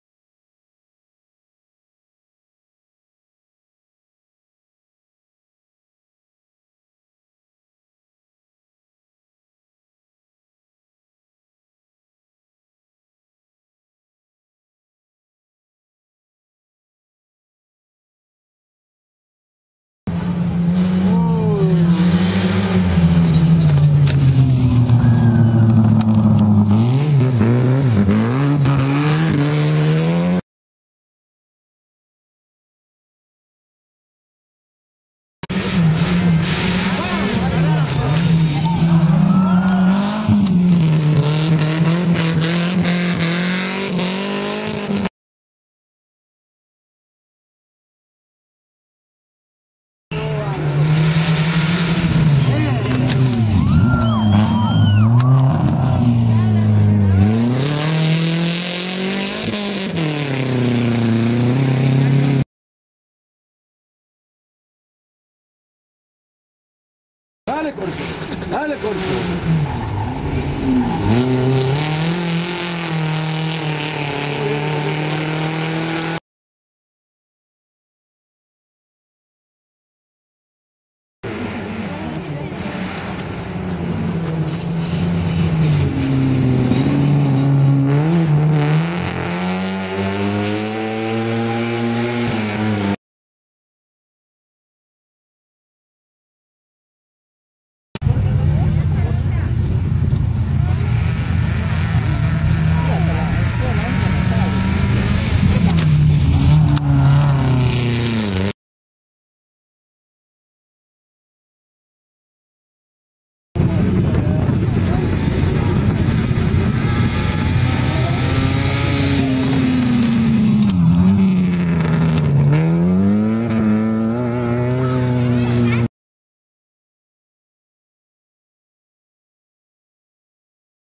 Rally de Corrientes 2012.Los Misioneros.
Todas las imágenes de la actuación de los representantes de Misiones en la 1ra. Fecha del Rally Entrerriano que tuvo como epicentro la ciudad de Corrientes.